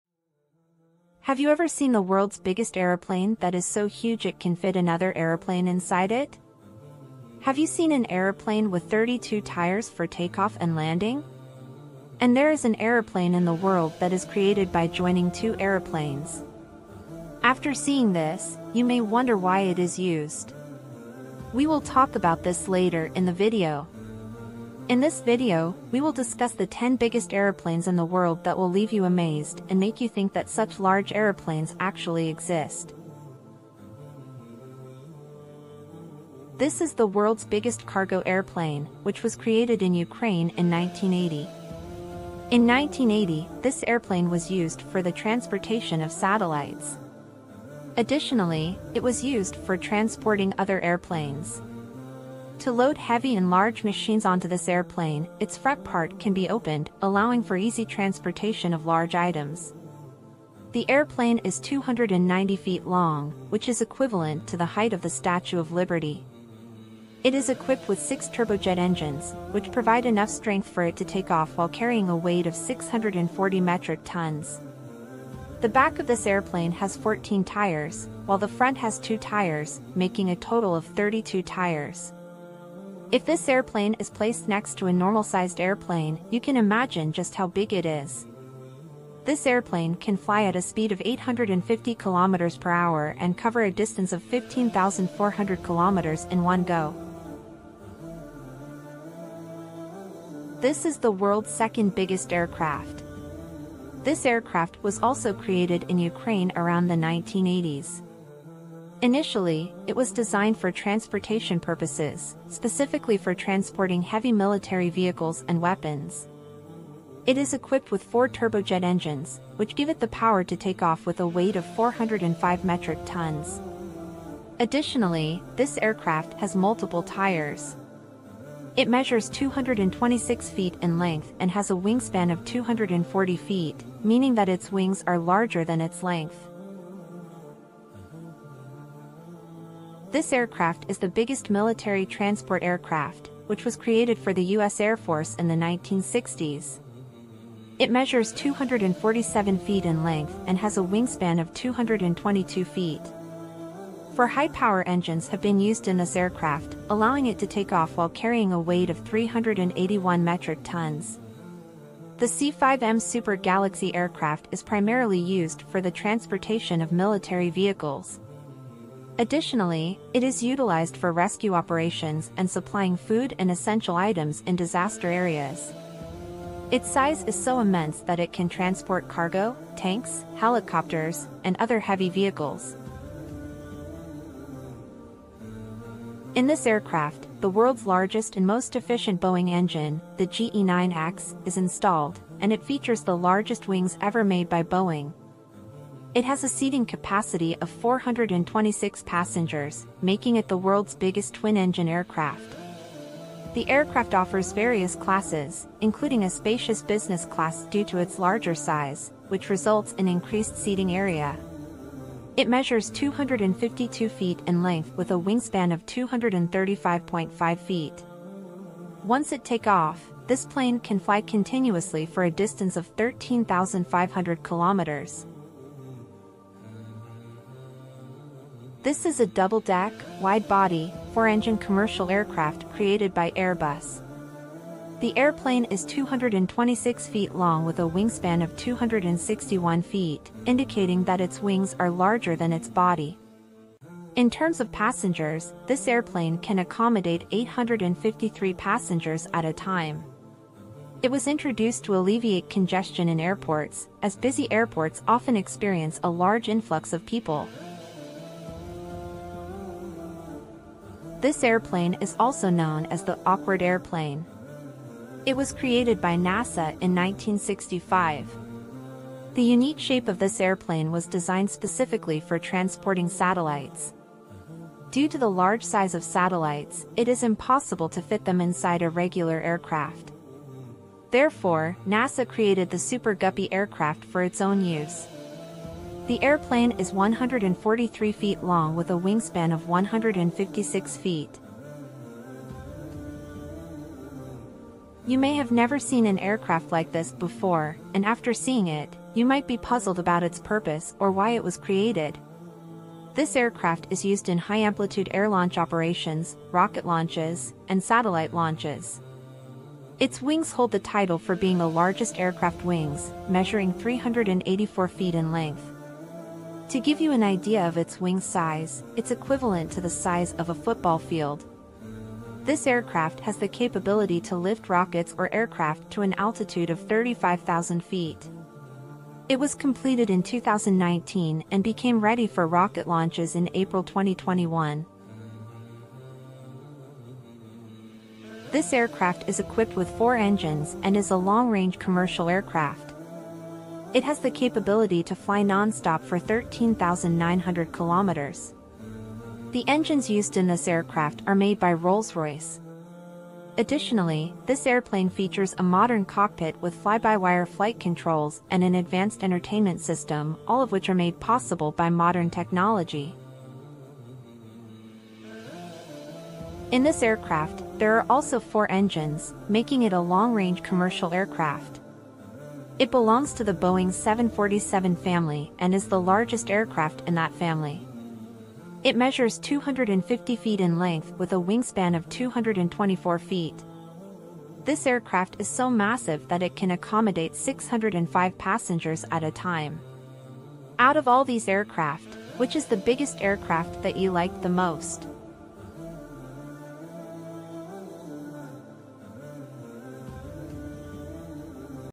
10 Biggest Airplanes In The World
10 Biggest Airplanes In The World

Prepare to be amazed as we take you on a journey through the incredible world of aviation. In this thrilling video, we unveil the top 10 largest airplanes ever built. From massive cargo carriers to jaw-dropping passenger planes, these flying giants will leave you in awe of human engineering and innovation. Join us as we explore the fascinating features and mind-boggling statistics of these colossal aircraft. Buckle up and get ready to witness the true wonders of flight!

00:00 Intro
00:34 Antonov AN-225 MRYA
01:38 Antonov AN-124 Ruslan
02:18 C-5M Super Galaxy
03:04 Boeing 777-9X
03:47 Airbus A380-800
04:19 Aerospace Line Super Guppy
04:53 Stratolaunch
05:36 Airbus A340-600
06:04 Boein 747-8